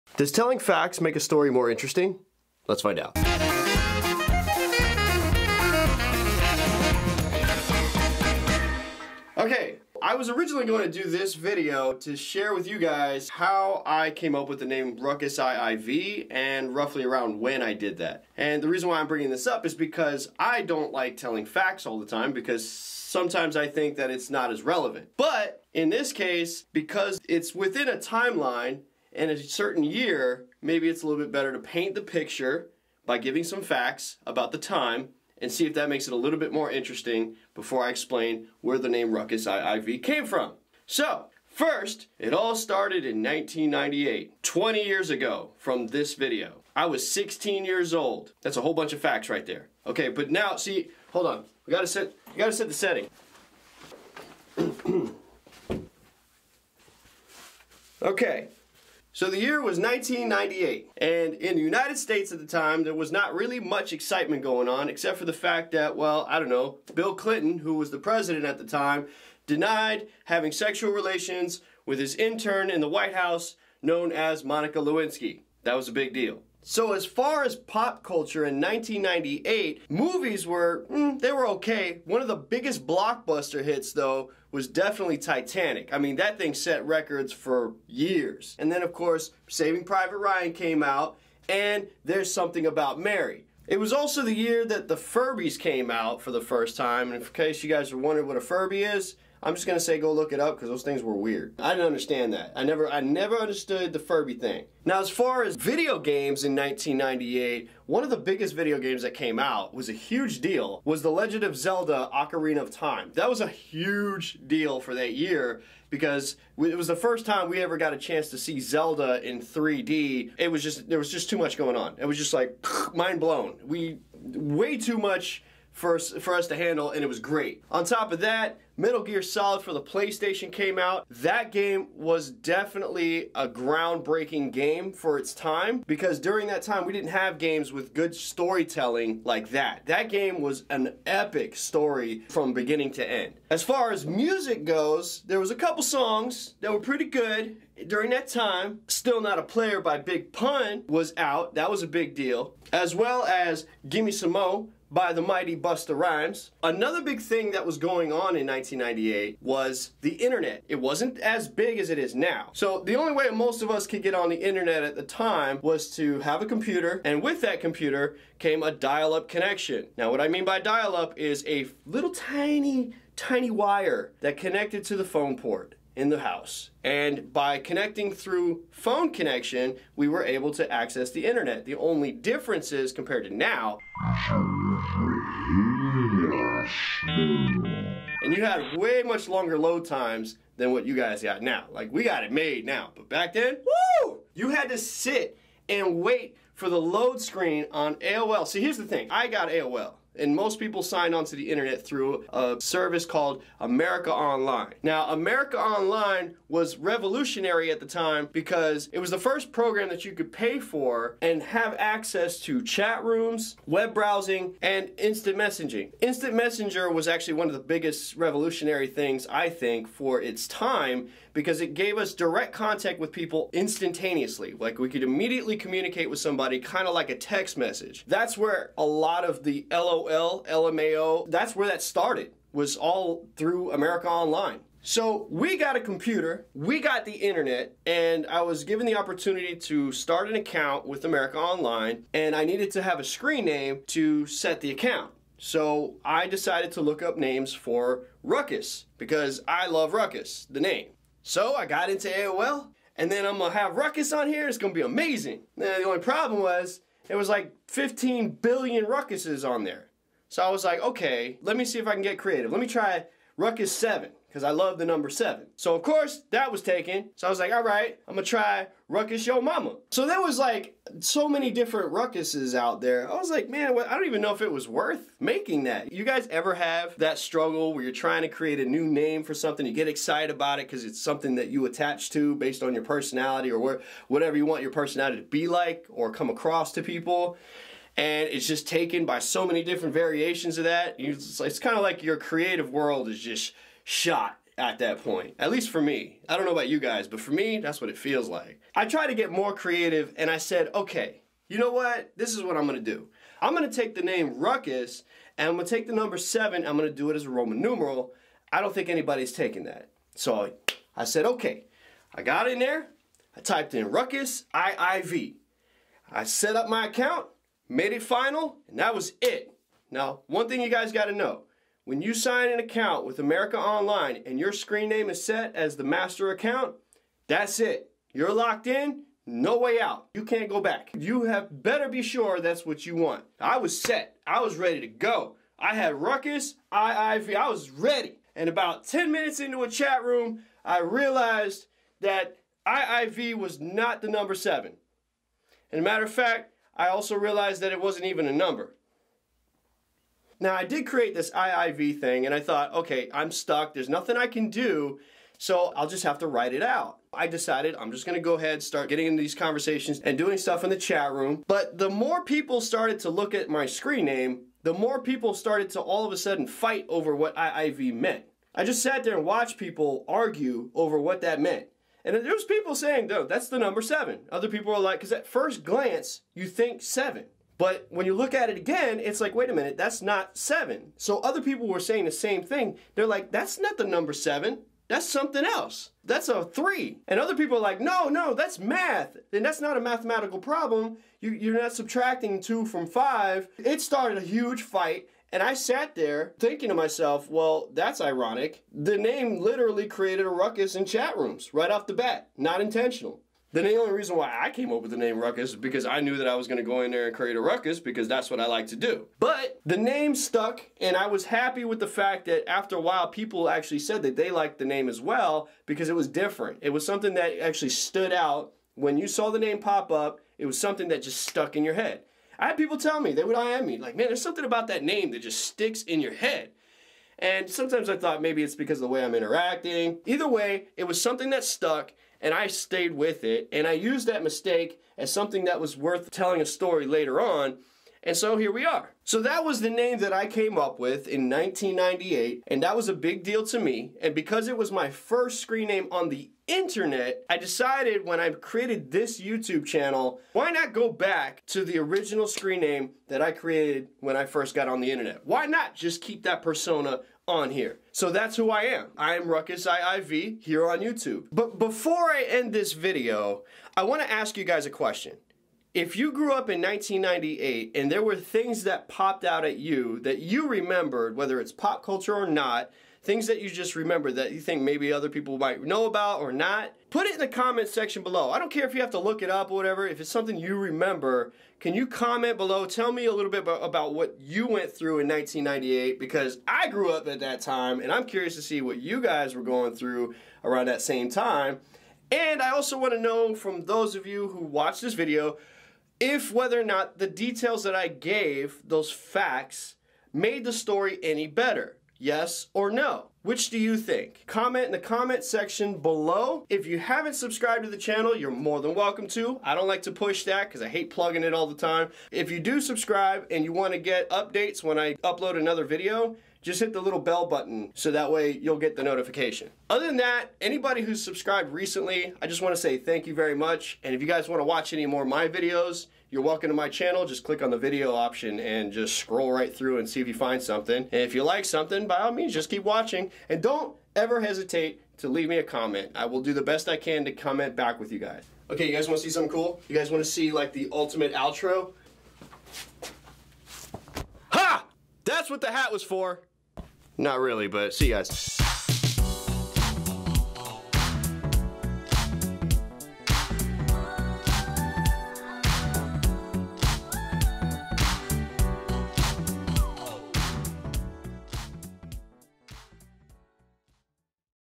How I came up with Ruckus iiV in 1998
Remember when America Online was a thing in the 90's? Yeah, that's where this story about how I came up with a screwed up screen name like Ruckus iiV begins.